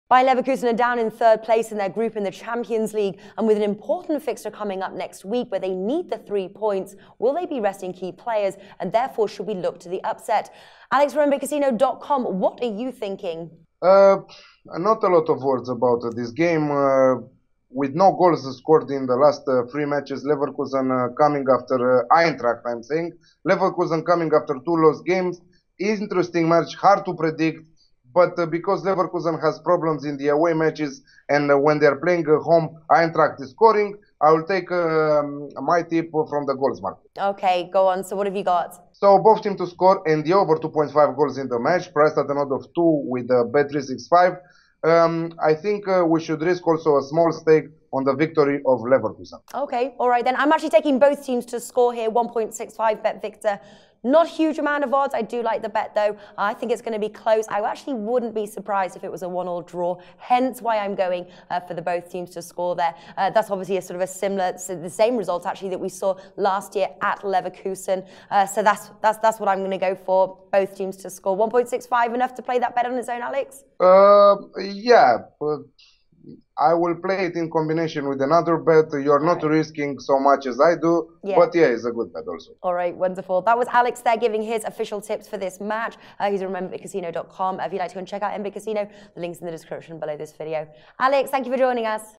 Eintracht Frankfurt vs Bayer Leverkusen 21/11/15 | Bundesliga Match Betting
Sportsbook Review's Natalie Rydstrom previews and offers free betting predictions for the week 13 German Bundesliga football match Eintracht Frankfurt vs Bayer Leverkusen playing on Saturday 21st November 2015 and highlights the betting odds with guest Alex from mBitCasino.com and Facebook